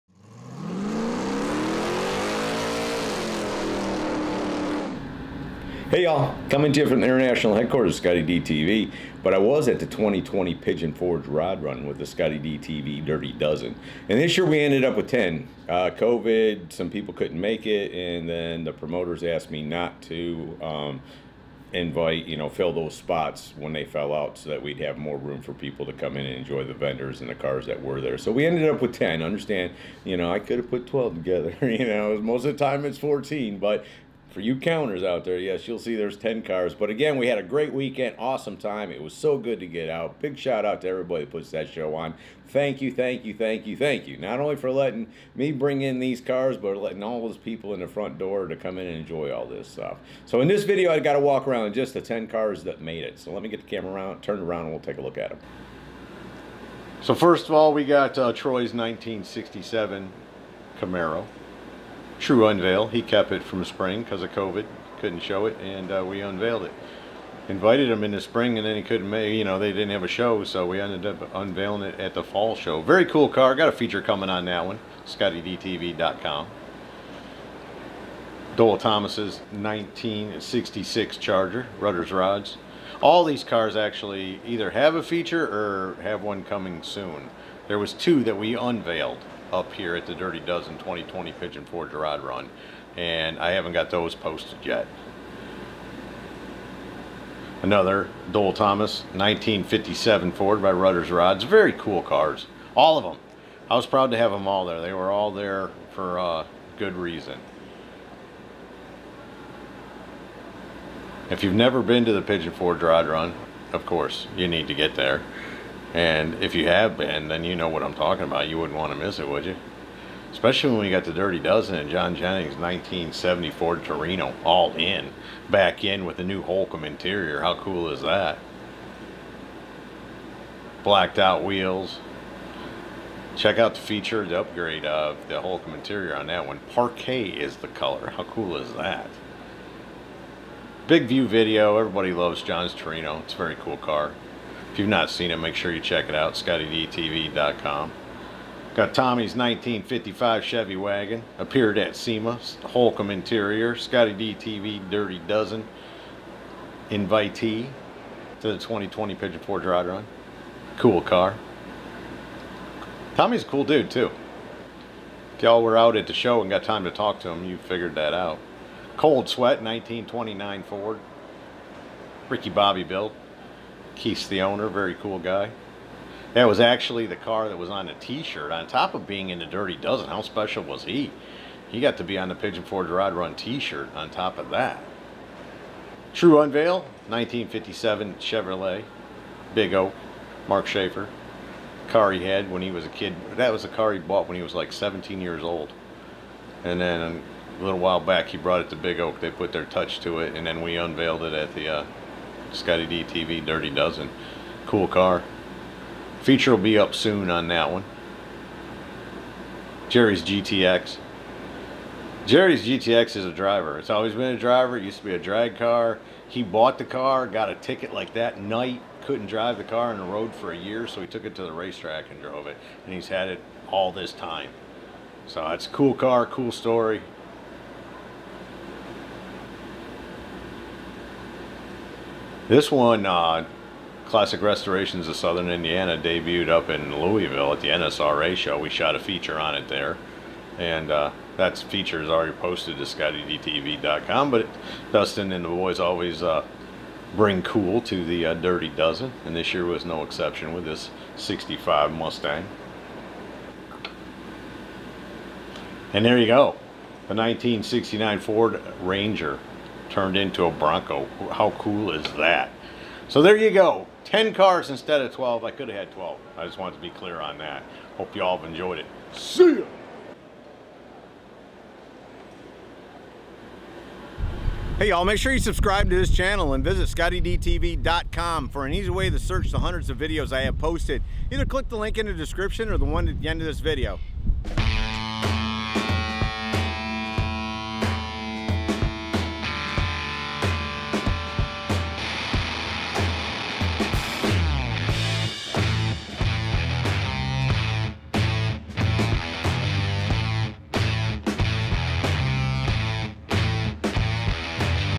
2020 ScottieDTV Dirty Dozen Walkthrough 2020 Pigeon Forge Rod Run
In this video from the 2020 ScottieDTV Dirty Dozen that is part of the 2020 Pigeon Forge Rod Run held at the LeConte Center Pigeon Forge Tn I do a walkthrough of the Dirty Dozen display...For the past several years ScottieDTV has been able to personally invite 12 cars that the channel feels would be something everyone would want to see to the Pigeon Forge Rod Run.. And the 2020 ScottieDTV Dirty Dozen was something to see for sure...This year with the COVID going on there were only 10 that were displayed in the front row of the LeComte Center..It is a big honor to have such cool vehicles representing the ScottieDTV brand...Even though the Dirty Dozen was only 10 for 2020 it was a collection of cool that was worth the price of admission to the 2020 Pigeon Forge Rod Run...It is always important for Me to mix things up when I'm putting together the list of cars and trucks I will invite to the ScottieDTV Dirty Dozen every year. Ford..Chevrolet. And Mopar are all on display... There are Sedans...Coupes. A wagon...A Fastback. Even if pickup that was turned into a old school Bronco...I know Your going to agree...The ScottieDTV Dirty Dozen from the 2020 Pigeon Forge Rod Run is something to see for sure...Check it out!!!

Coolest Cars On The Web!!! Coolest Trucks On The Web!!